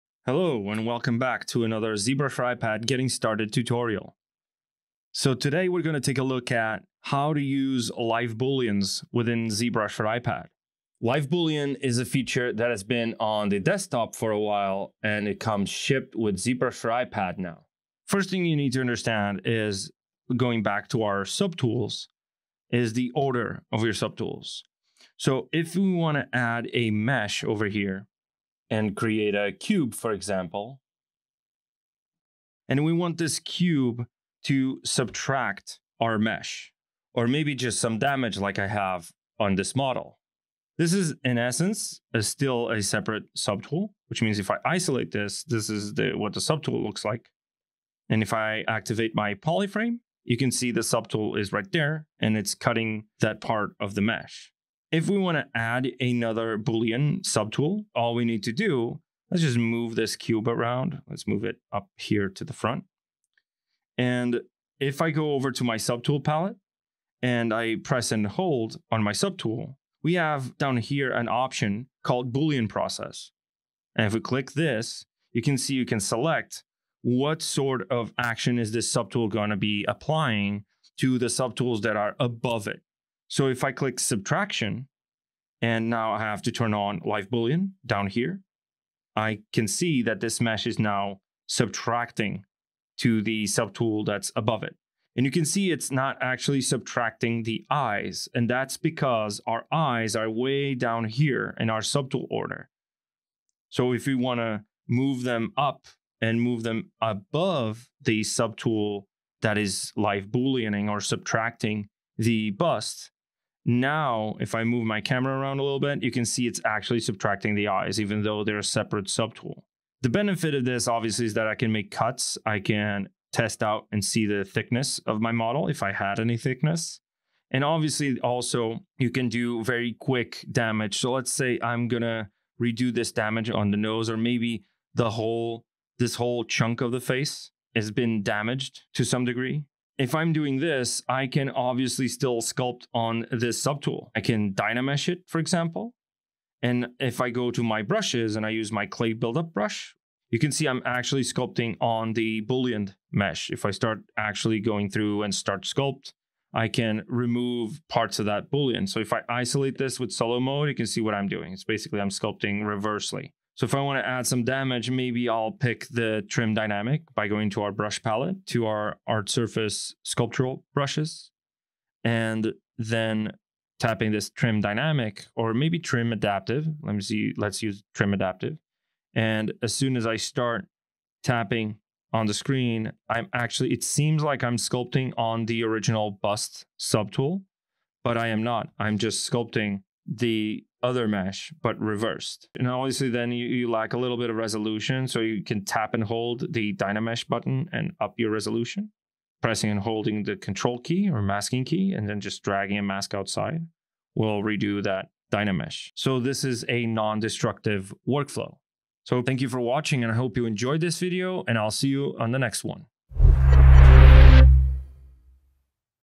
19 - ZBrush for iPad - Live Boolean
Let’s talk about how to be nice, kind and totally not destructive with our models by using Live Booleans in ZBrush for iPad.

for more info on ZBrush for iPad.